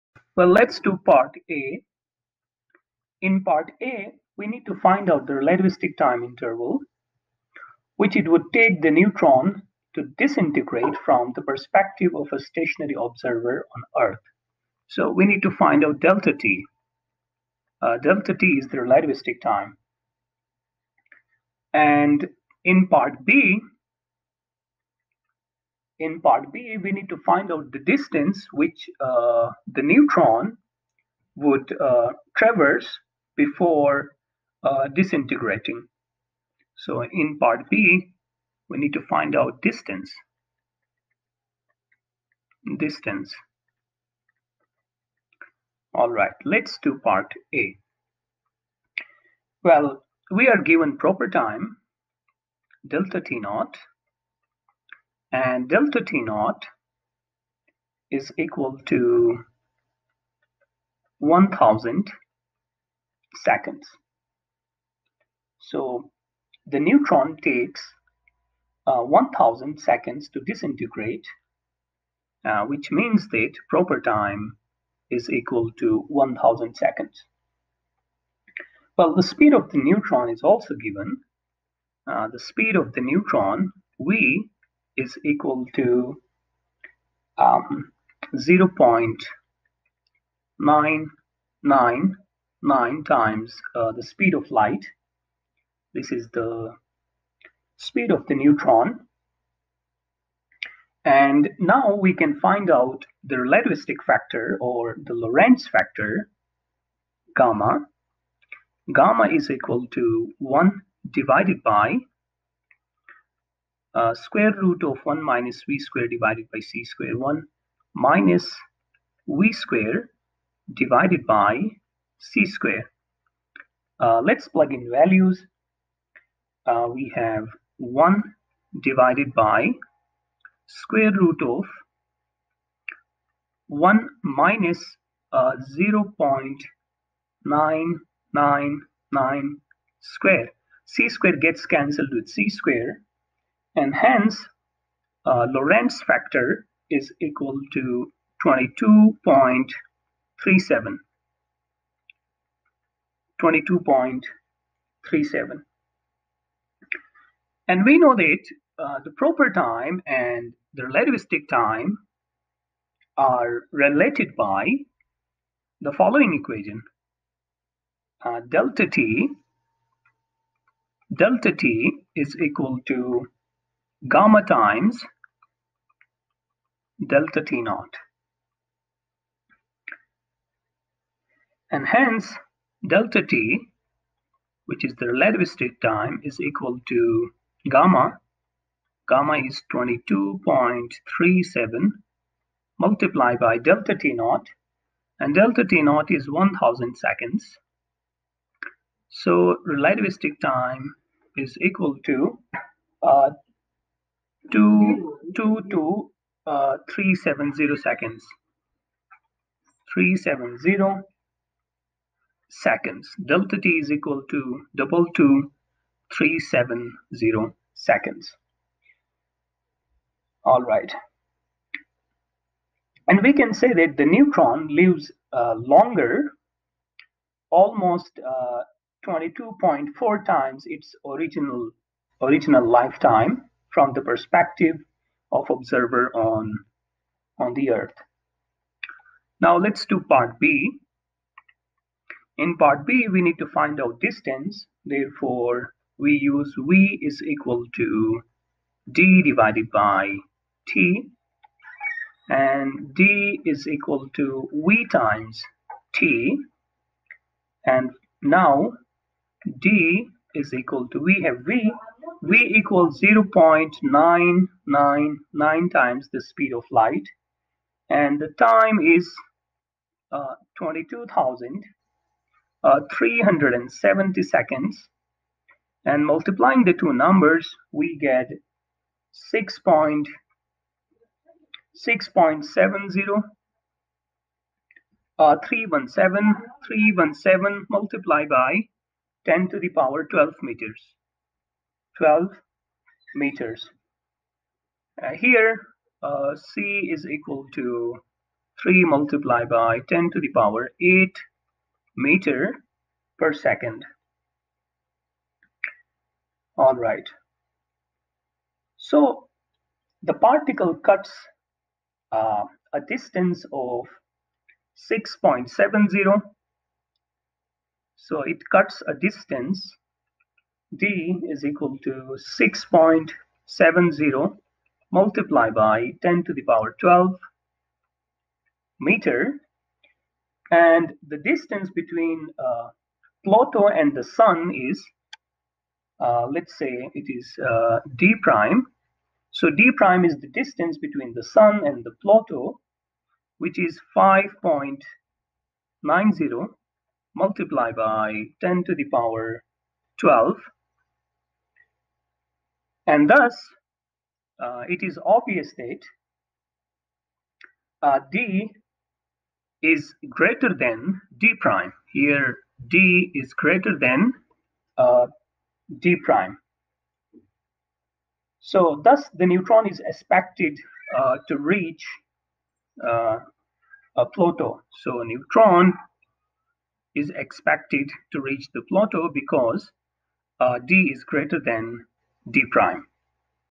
Extending life A free neutron lives about 1000 s before transforming into an electron and a proton
Extending life A free neutron lives about 1000 s before transforming into an electron and a proton. If a neutron leaves the Sun at a speed of , (a) how long does it live according to an Earth observer (b) Will such a neutron reach Pluto ( from the Sun) before transforming Explain your answers.